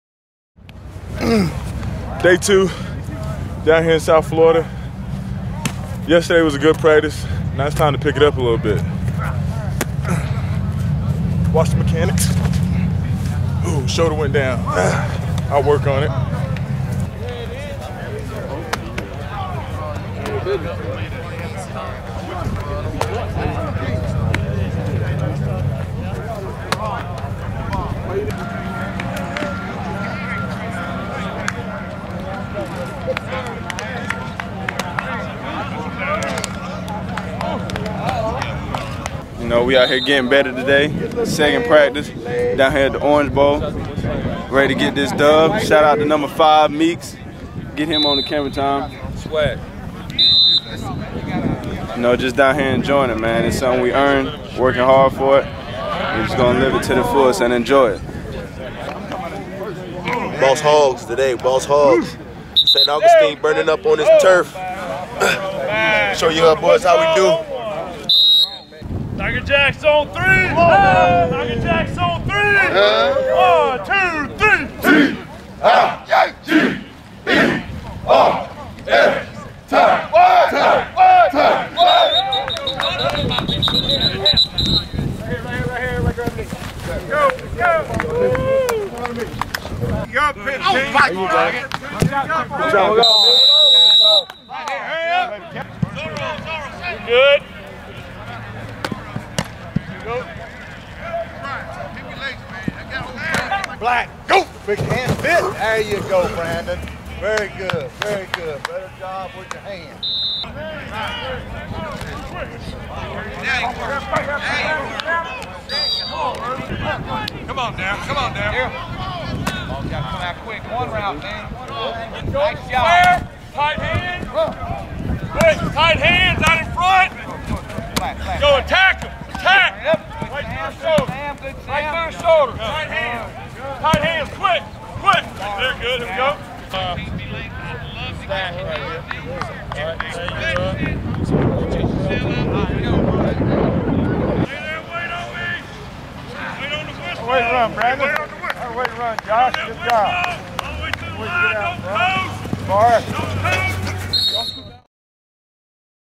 Clemson Orange Bowl Report Dec. 28
Highlights from day two of Discover Orange Bowl preparations for the Clemson Tigers.